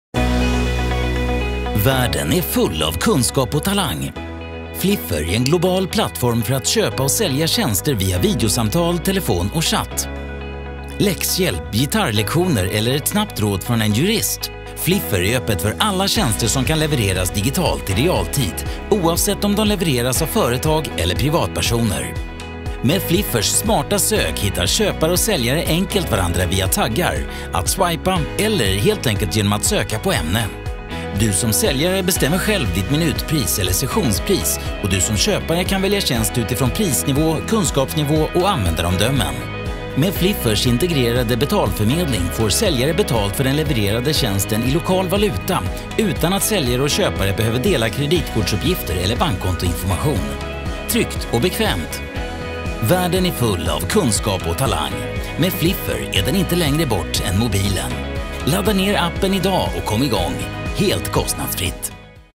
Fliffr Svenska Explainer video 1 min
Världen är full av kunskap och talang. Fliffr är en global plattform för att köpa och sälja tjänster via videosamtal, telefon och chatt. Behöver du läxhjälp, gitarrlektioner eller ett snabbt råd från en jurist?
Fliffr är öppet för alla tjänster som kan levereras digitalt i realtid oavsett om de levereras av företag eller privatpersoner.

Med Fliffrs smarta sök hittar köpare och säljare enkelt varandra via taggar, att swipa eller helt enkelt söka på ämne. Du som säljare bestämmer själva ditt minutpris eller sessionspris och du som köpare kan välja tjänst utifrån, prisnivå, kunskapsnivå och användaromdömen. Med Fliffrs integrerade betalförmedling får säljare betalt för den levererade tjänsten i lokal valuta utan att säljare och köpare behöver dela kreditkorsuppgifter eller bankontoinformation. Tryggt och bekvämt!

Fliffr-tekniken kan också integreras på en webbsajt eller app. På så sätt kan företag och organisationer leverera tjänster till sina kunder direkt via sina egna kanaler – på samma sätt som de använder en e-handelsplattform för att sälja fysiska produkter.

Världen är full av kunskap och talang. Med Fliffr är den inte längre bort än mobilen. Ladda ner appen idag och kom igång – helt kostnadsfritt!